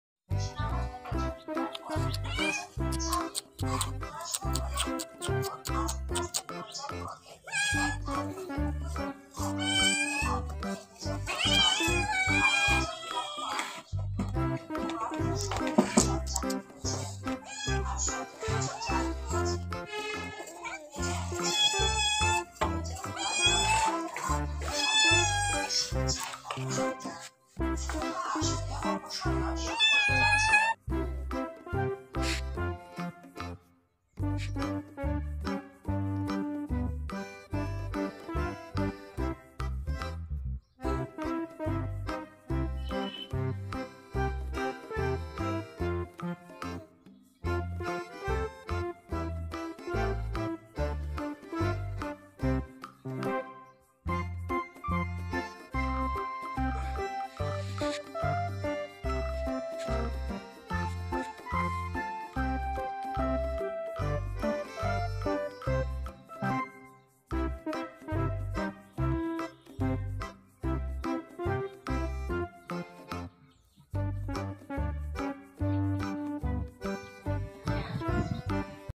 Hungry Cats Want Their Food, I Give Them Fish Fry
Hungry cats want there feed। I give them fish fry.